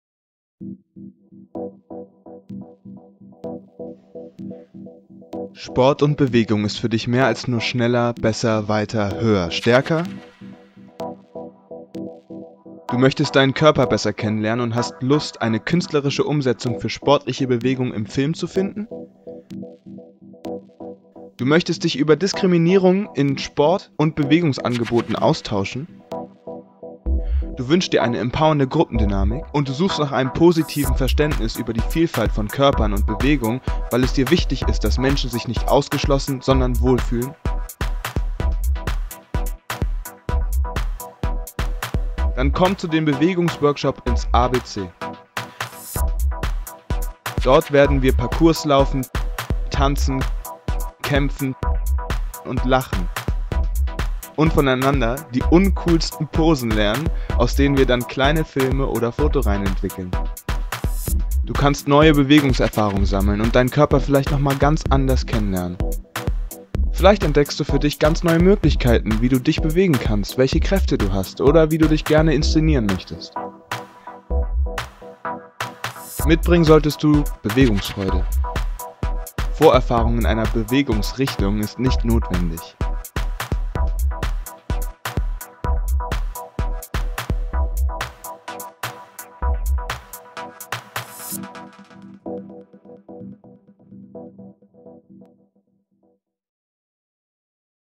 SHIFT IT - Bewegung.Medien.Kunst Trailer
Sport und Bewegung ist für dich mehr als nur schneller besser weiter? Du möchtest deinen Körper besser kennenlernen, und hast Lust eine künstlerische Umsetzung für sportliche Bewegungen zu finden? Du möchtest dich über Diskriminierung in Sport und Bewegungsangeboten austauschen? Du wünschst dir eine empowernde Gruppendynamik? Und du suchst nach einem positiven Verständnis über die Vielfalt von Körpern und Bewegung weil es dir wichtig ist, dass Menschen sich nicht ausgeschlossen, sondern wohlfühlen?

Dann komm zu den Bewegungs-Workshops ins ABC. Dort werden wir tanzen und kämpfen, Parkour laufen, Schreien und Lachen und voneinander die (un)coolsten Posen lernen, aus denen wir dann kleine Filme oder Fotoreihen entwickeln. Du kannst neue Bewegungserfahrungen sammeln und deinen Körper vielleicht nochmal ganz anders kennenlernen. Vielleicht entdeckst du für dich ganz neue Möglichkeiten, wie du dich bewegen kannst, welche Kräfte du hast, oder wie du dich gerne inszenieren möchtest.
Mitbringen solltest du Bewegungsfreude. Vorerfahrung in einer Bewegungsrichtung ist nicht notwendig.

SHIFT IT wird finanziert von der Aktion Mensch.